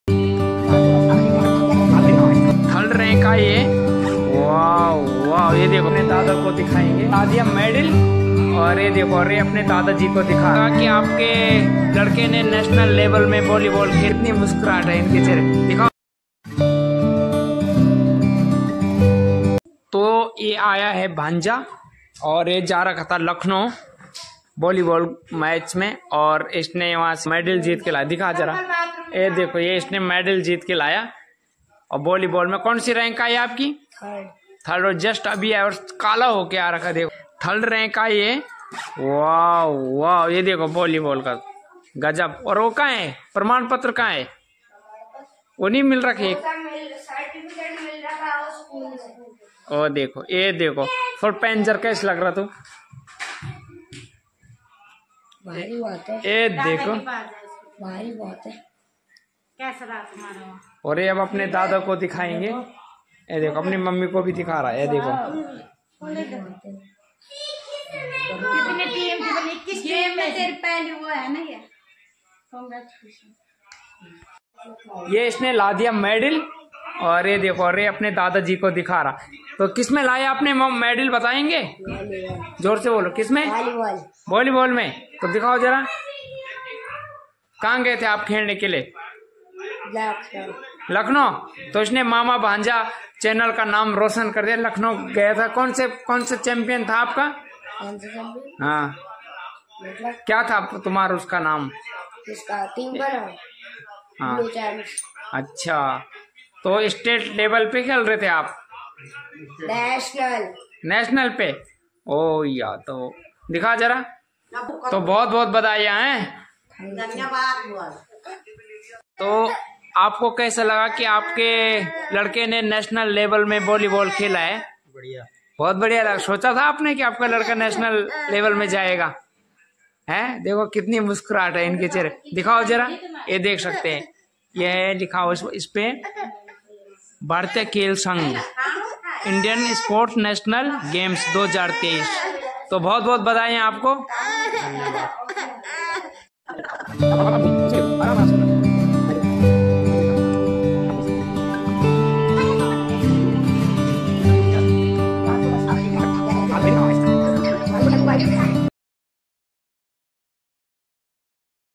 मामा भांजा ब्लॉग का नाम रोशन || volleyball india sports national games 2023
SHRI GURU NANAK H/S PUBLIC SCHOOL

Mayank Thapliyal

Our Volleyball team have earned a place in the Indian Sports National Games 2023, and we celebrate their achievements with pride. We encourage them to showcase their prowess in Lucknow from October 29th to the 31st.